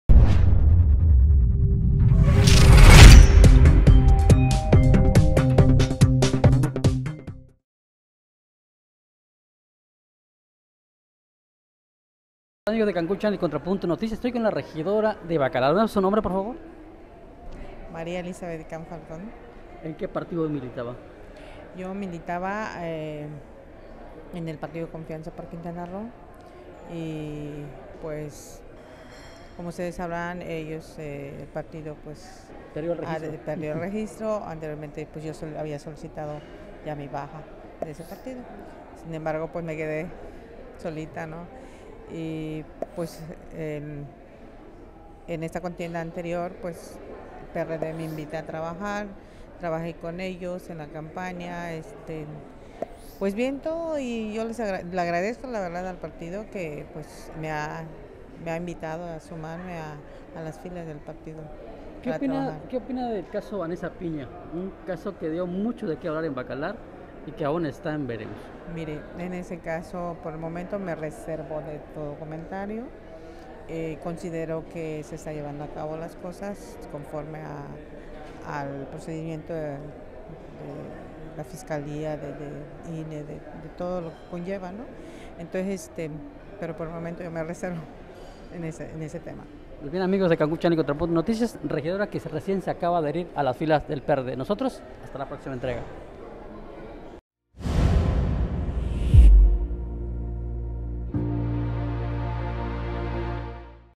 REGIDORA DE CONFIANZA POR QUINTANA ROO "SE PASA" AL PRD
*Ante la desaparición del partido quintanarroense, la regidora Elizabeth Campa, de Bacalar, se adhiere al PRD; del caso "Vannesa.